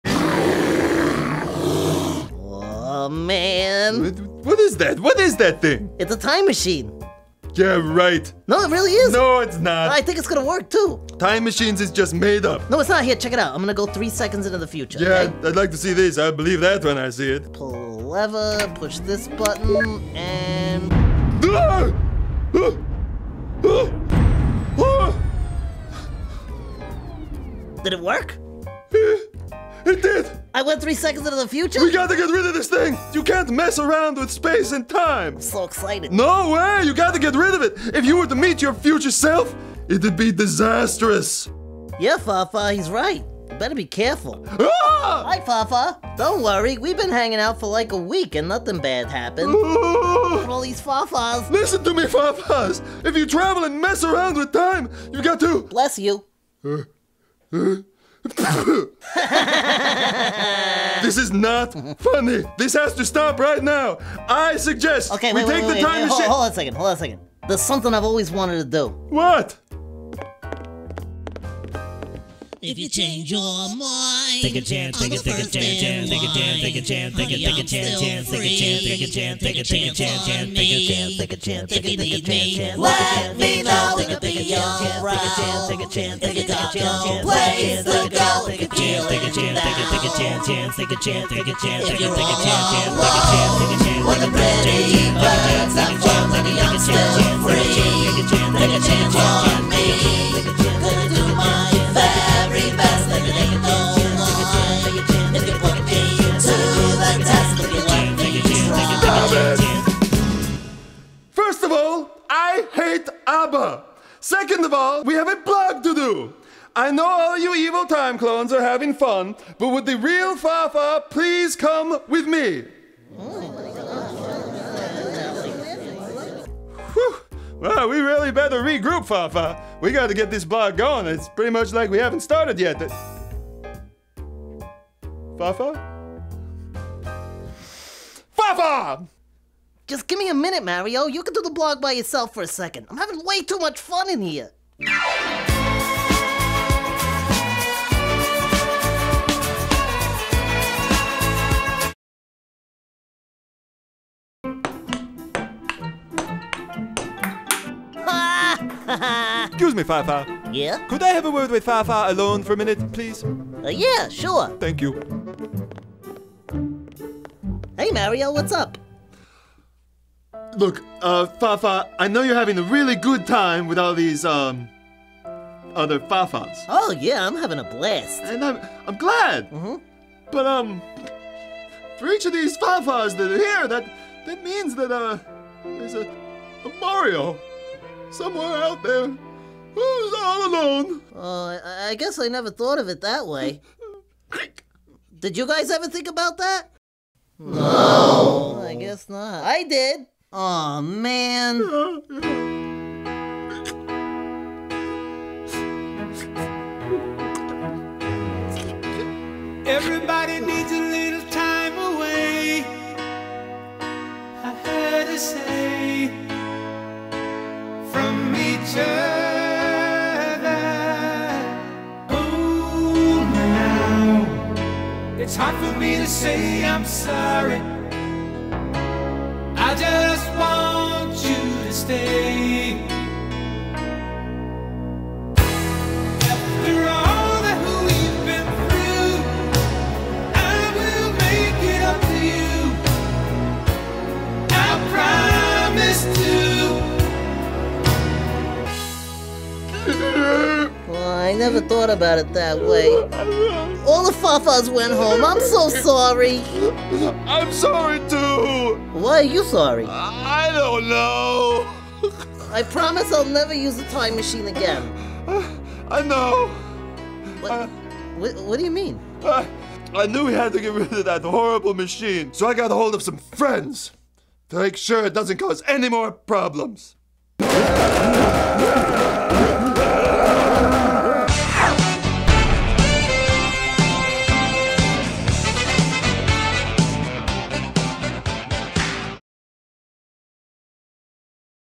Time Machine
Fafa builds a time machine.